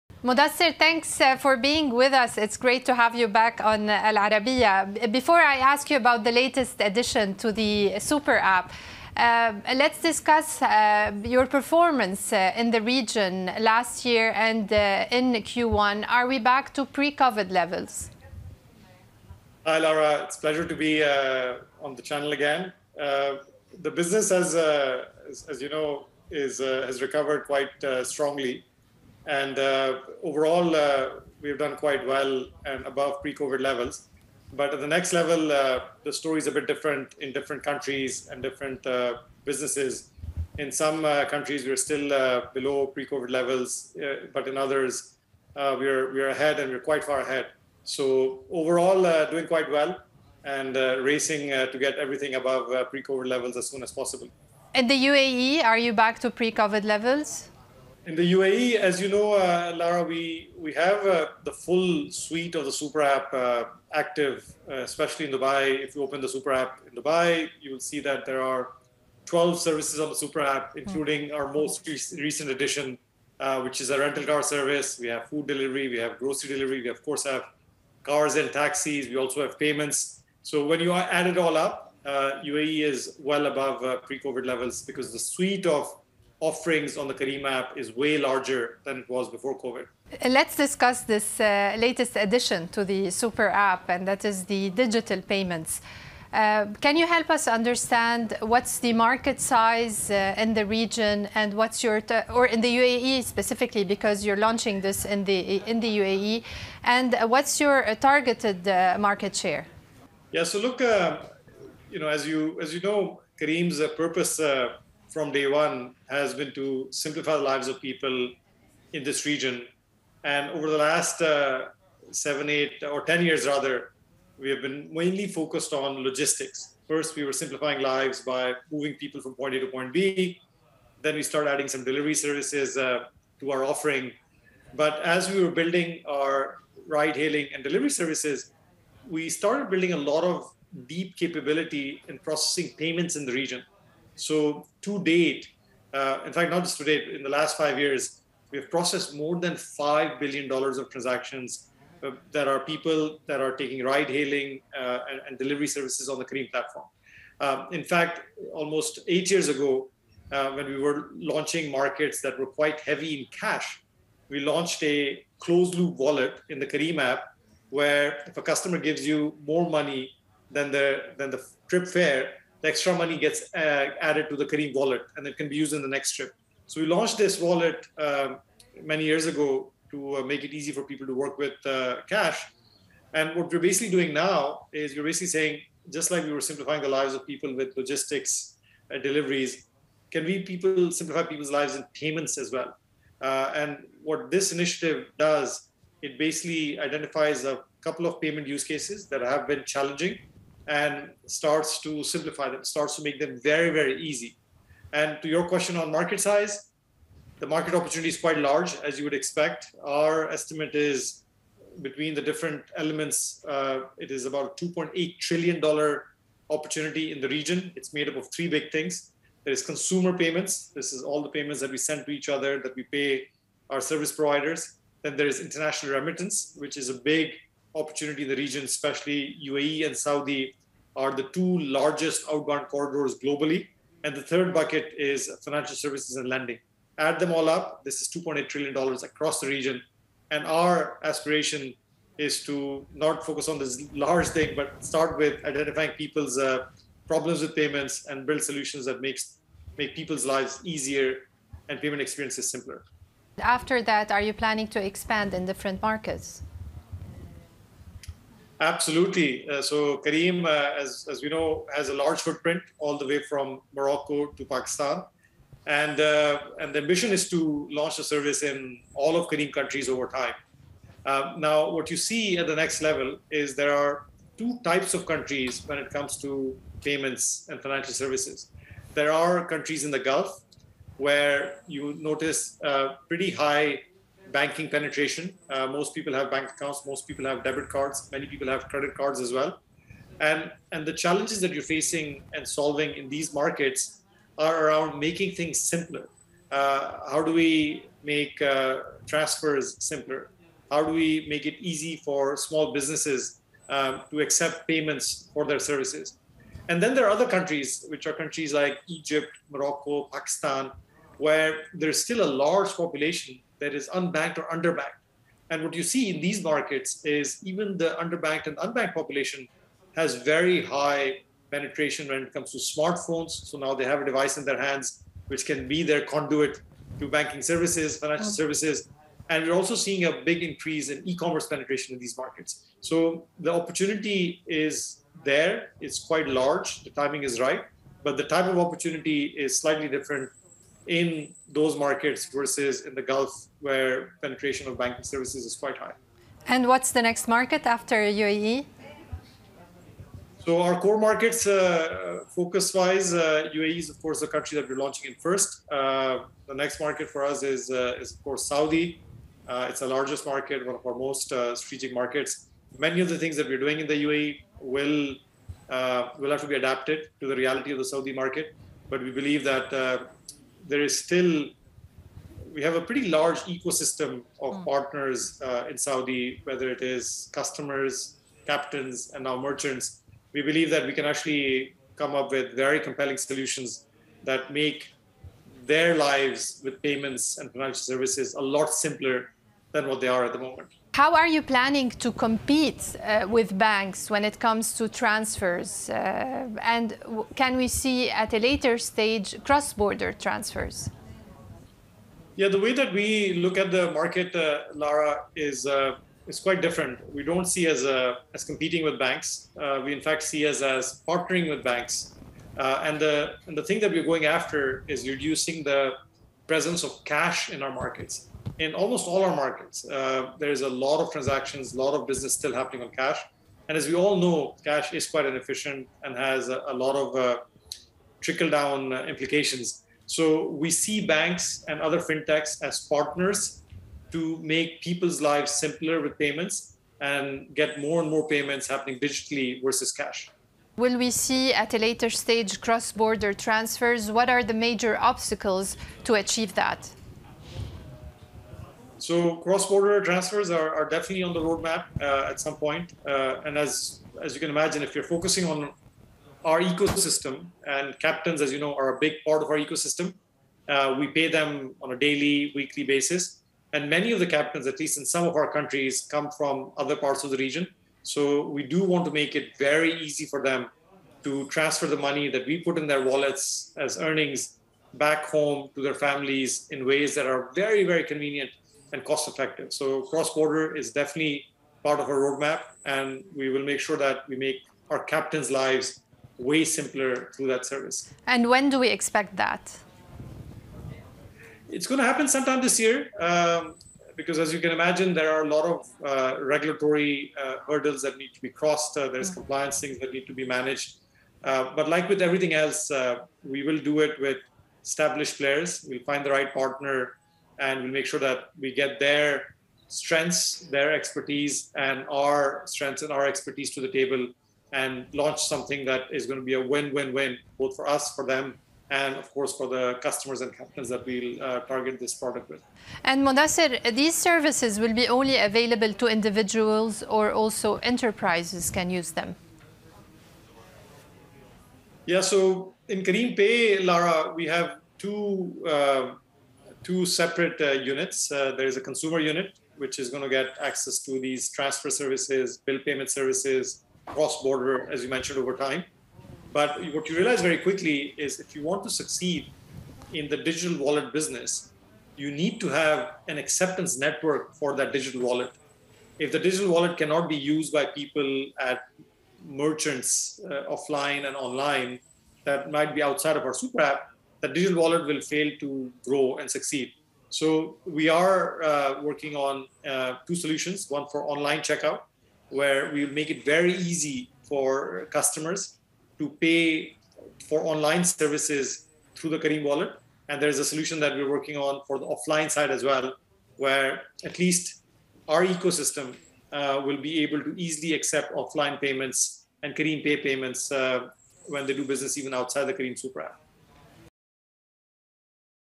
Full interview with Careem Founder and CEO Mudassir Sheikha
In an interview with Al Arabiya, Careem CEO Mudassir Sheikha  says the UAE-bred ride-hailing service will expand its latest fintech venture, digital payments, into Saudi Arabia.